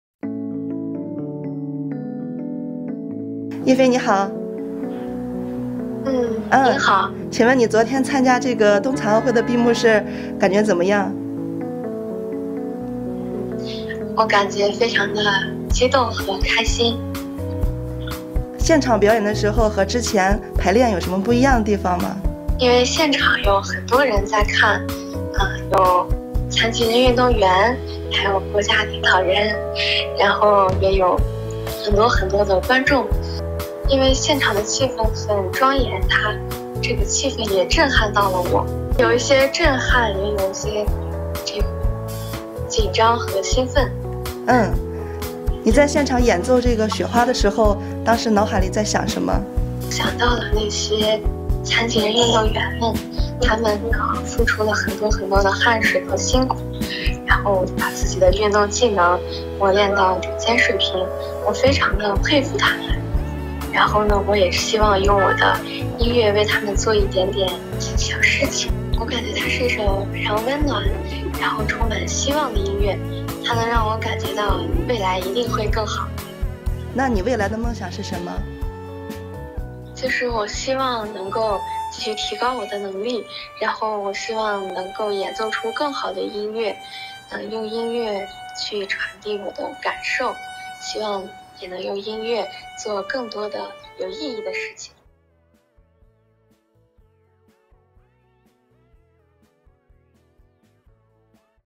Visually impaired violinist touches hearts at closing ceremony of Beijing Winter Paralympics
Sixteen-year-old visually impaired violinist Ma Yifei touched the hearts of many at the closing ceremony of the Beijing 2022 Paralympic Winter Games. Xinhua talks to her about her music, her life and her hope.